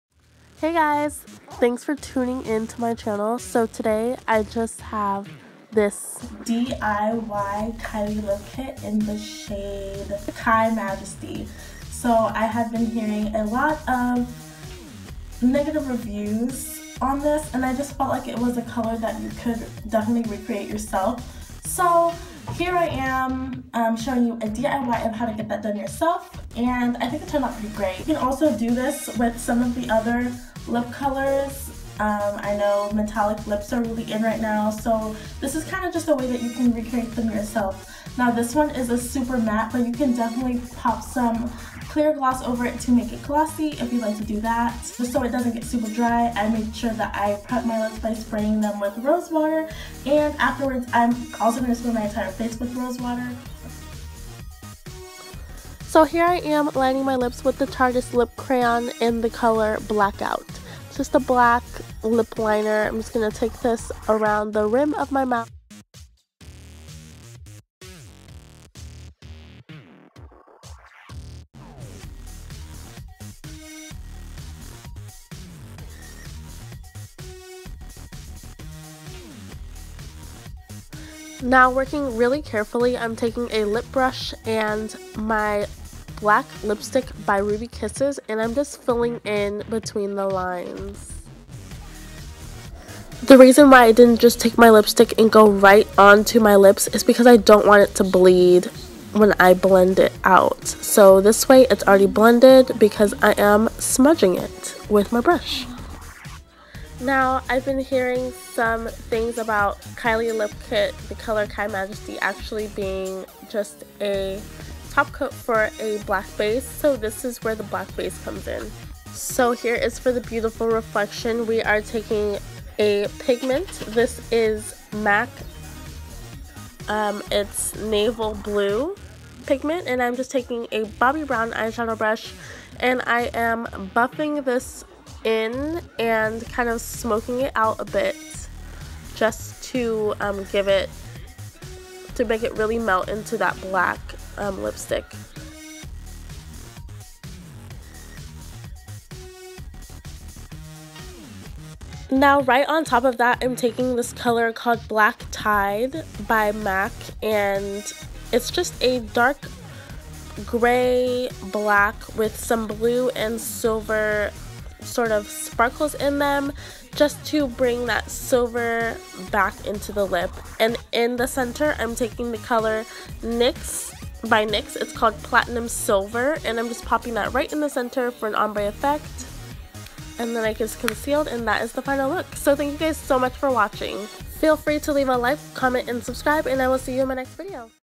DIY KY MAJESTY lip Kit!
Please remember to gently exfoliate and moisturize your lips prior to put on lipstick ever! Let me know what you want to see next? Hope you enjoyed!

Details ❤️

Eyes: MAC pro long wear paint pot "soft ochre"
BH cosmetics "red"
Bobbi brown "burgundy"
Tartelette palette in bloom
MAC shadow "shadowy lady"
MAC false lash maximizer
The color workshop mascara

Brows: Bobbi Brown corrector "very deep bisque
MAC "shadowy lady"

Face: Laura Mercier oil free supreme foundation "rich sienna"
MAC studio sculpt foundation "NC50"
Ben Nye translucent powder "topaz"
MAC powder blush "sketch"
MAC powder blush "blunt"
Bobbi brown skin weightless powder foundation "walnut 8"
MAC mineralize skinfinish "global glow"

Lips: Tartiest pencil "black"
          Ruby kisses lipstick "black"
          MAC pigment "naval blue"
          MAC shadow "black tie"
          NYX shadow "platinum silver"
          Rose water